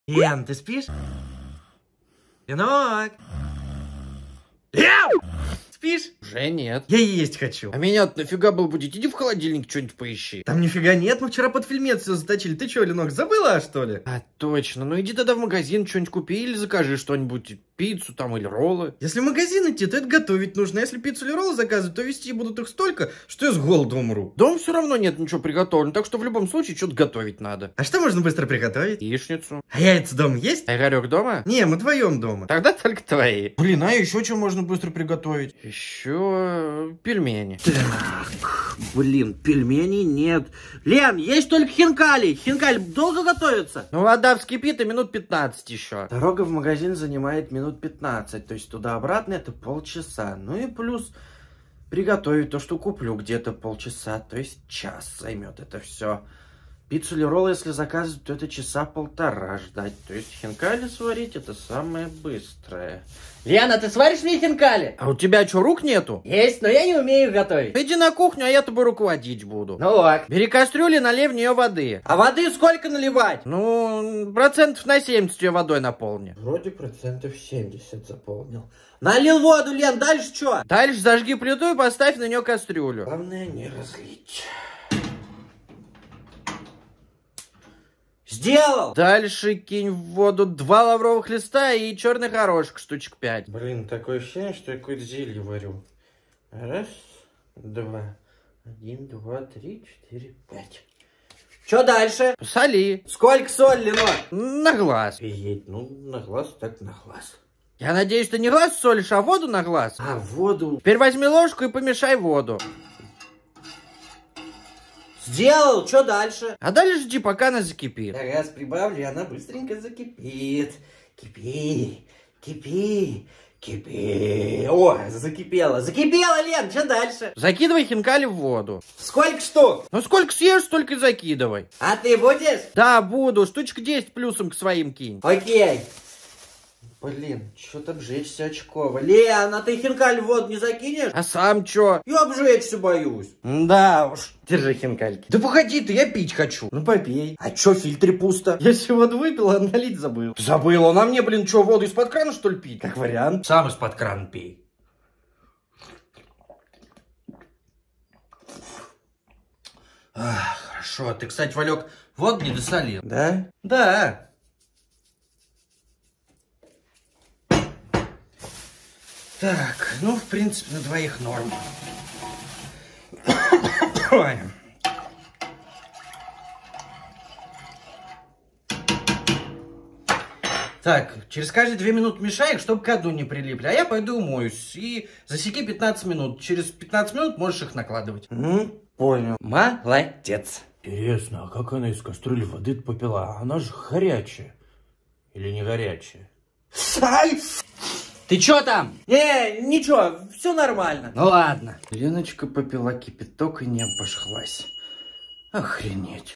СЕРИАЛ "ЖИЗНЬ БЕЗ РОДИТЕЛЕЙ" / 29 СЕРИЯ / ХОЧУ ЕСТЬ / GREY SHIP
Валёк хочет есть, но в доме ничего нет готового, смотрите видео до конца, чтобы узнать чем всё закончилось!

Видео → СЕРИАЛ "ЖИЗНЬ БЕЗ РОДИТЕЛЕЙ" / 29 СЕРИЯ / ХОЧУ ЕСТЬ / GREY SHIP